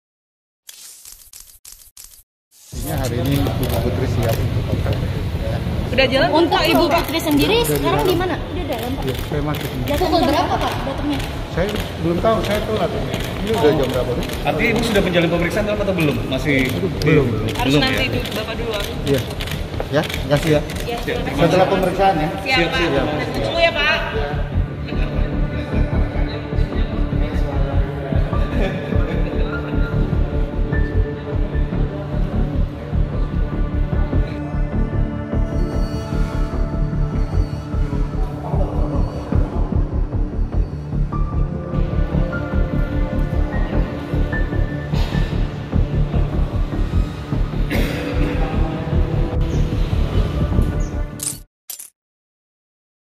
Tiba di Bareskrim, Kuasa Hukum Sebut Putri Candrawathi Siap Jalani Pemeriksaan Lanjutan
Putri Candrawathi lakukan pemeriksaan lanjutan di Bareskrim Polri pada Rabu (31/8) pagi. Istri Ferdy Sambo yang sudah berstatus tersangka akan diperiksa menggunakan metode konfrontir dengan keterangan tersangka lainnya.

Kuasa Hukum Putri Candrawathi, Arman Hanis datang untuk mendampingi pemeriksaan. Arman menyebut Putri dalam kondisi sehat dan siap mengikuti pemeriksaan lanjutan.